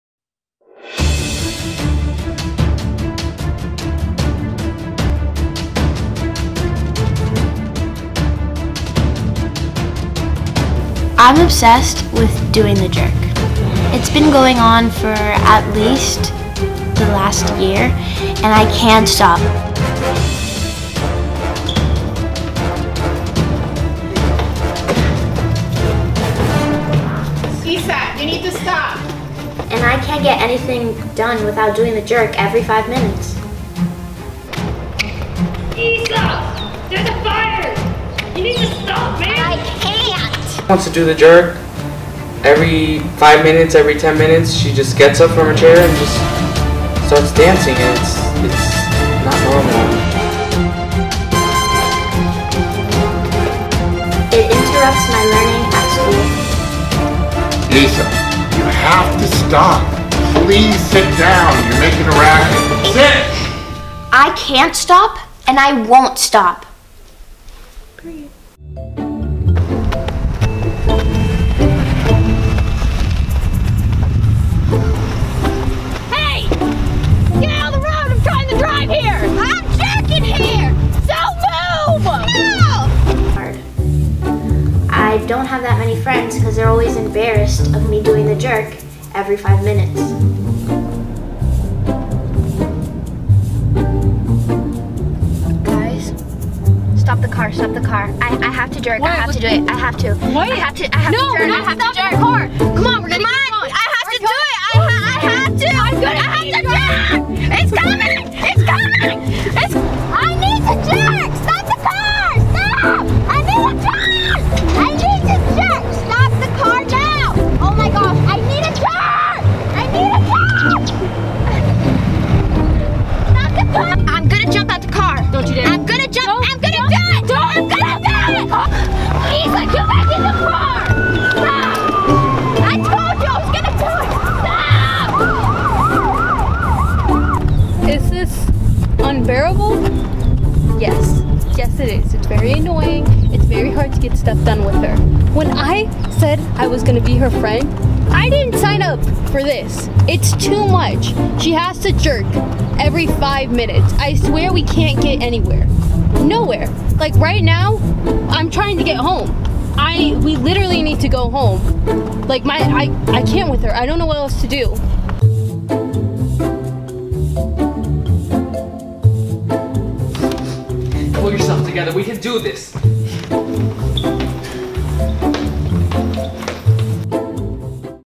My Strange Addiction {Parody}
Isa strange addiction is the jerking dance , we tried everything in our power to stop her but nothing has worked.
WBTV
By
Daniella Russo
Isabella Ballista
Kathryn Garner